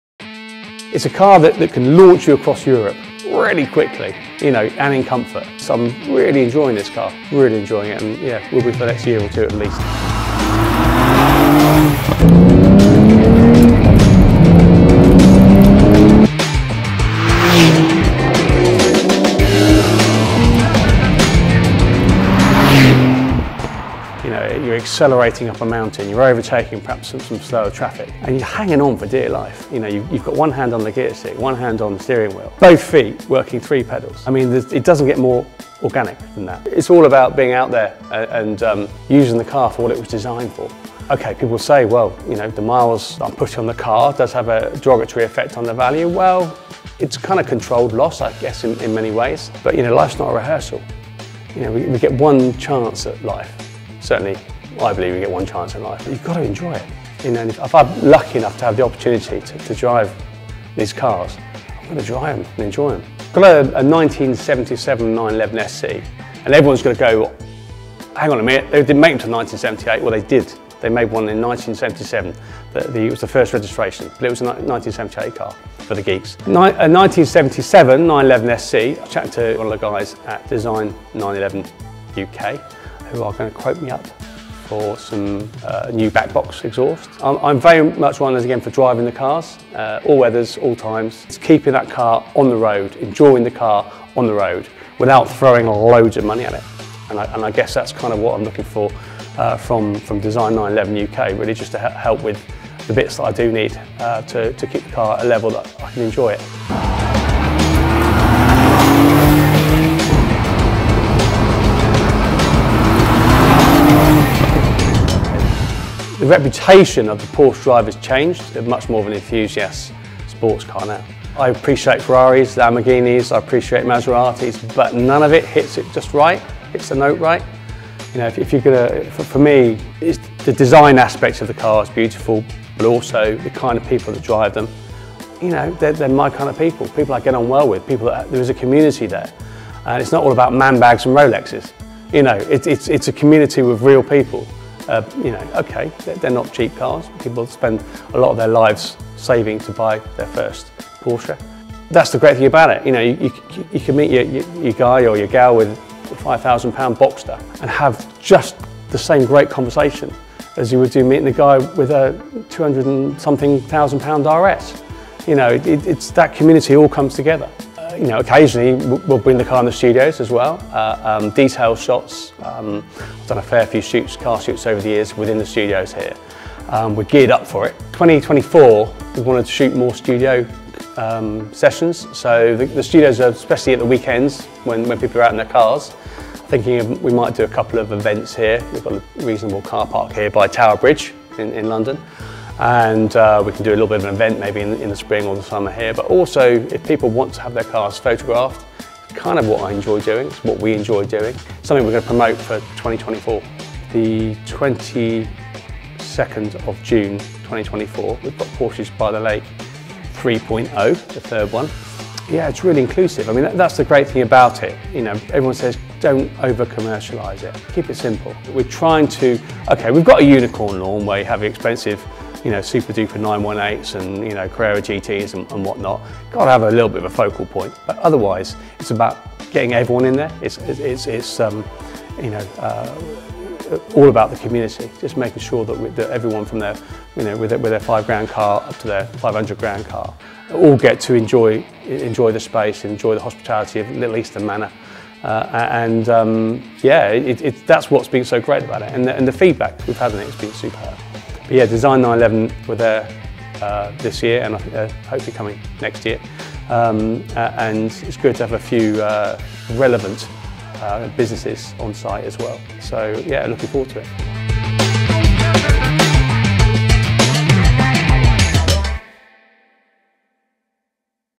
Porsche Owner Stories - Porsche 992 Sport Classic Unleashed
Owning and driving a car as beautiful as a 911 creates a dissonance. Because (a) when you’re driving a Porsche, clearly you can’t admire it as others do. Also, (b) you’re incrementally reducing the value with every tick of the odometer. So, which do you prioritise?

Buffing it lovingly with a Chamois leather, and trundling out to a country pub when you’ve checked the route for puddles, loose chippings and so on? Have you seen the price of a respray/new shocks etc.? Or do you jump in the thing at every possible opportunity, hit the pedals and to hell with the value? This is a car, not a museum piece!

We had a quick chat with James (@911_spy) owner of a manual marvel that is a 992 Sport Classic. He talks us through his experience with the car and why he is so passionate about driving.